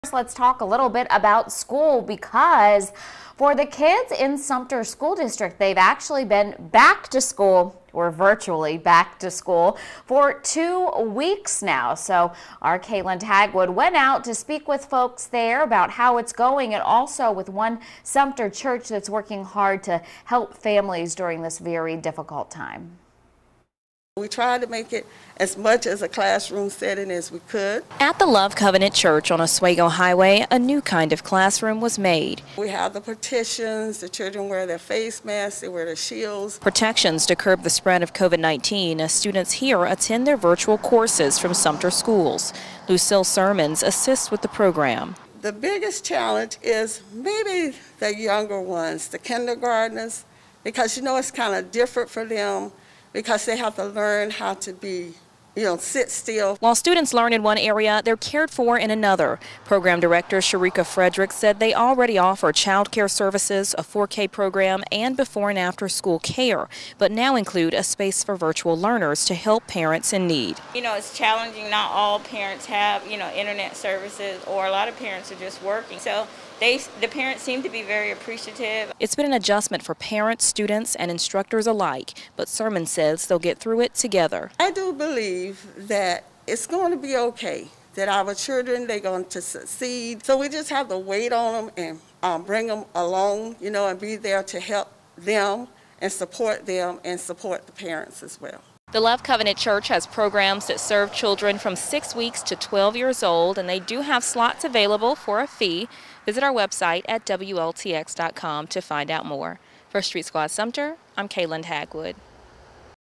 Virtual learning support centers in Sumter
In one area students learn and in another area they are cared for.